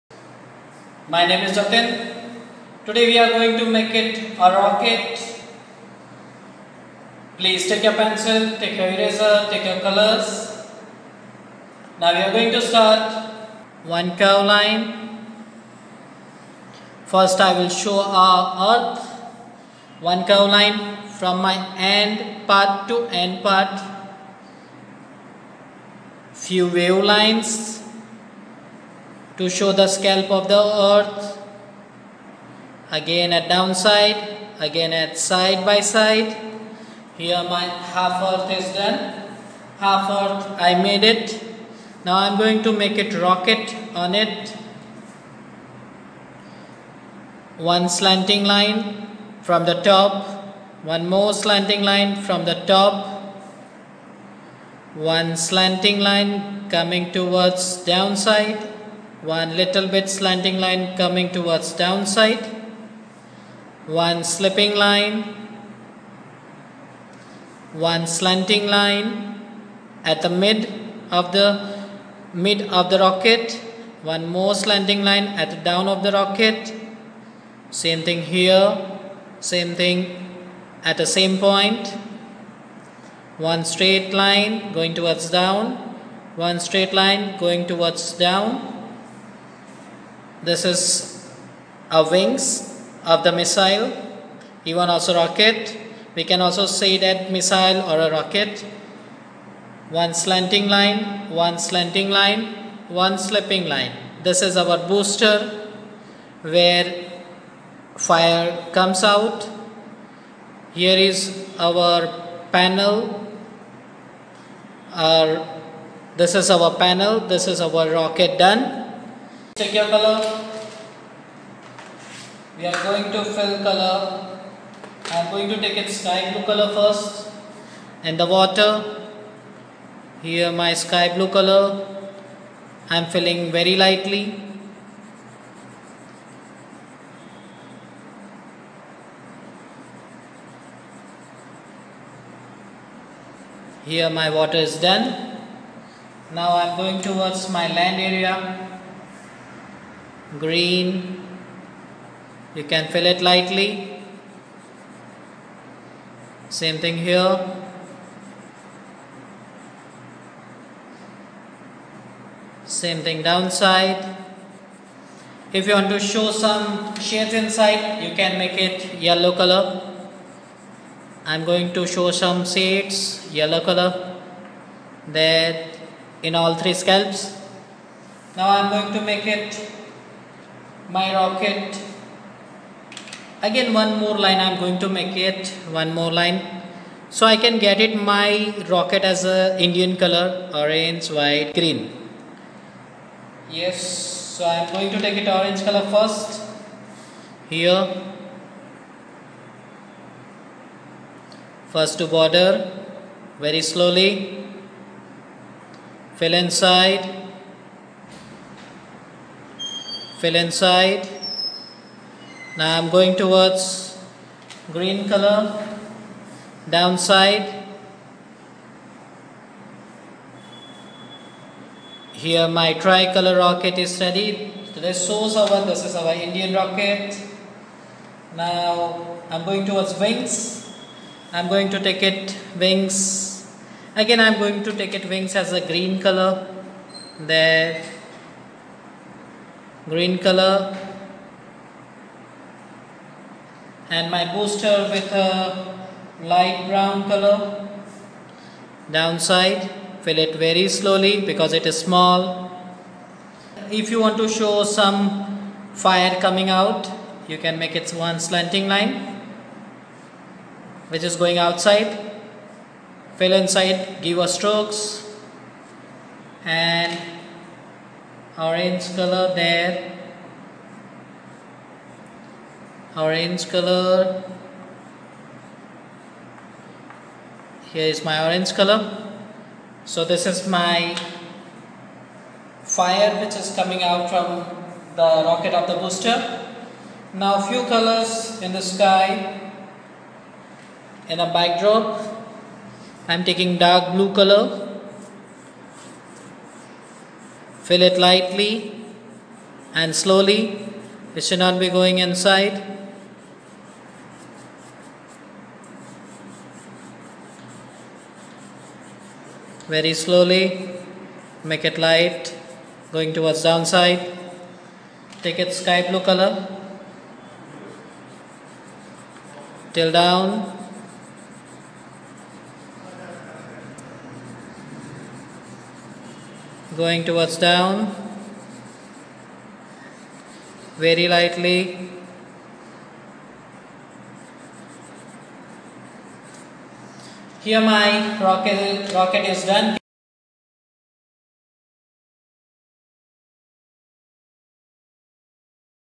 Easy and simple rocket drawing step by step/ with instructions
hello students. today i teach you how to draw a rocket using lines and shape. with instruction. kids special drawing (rocket)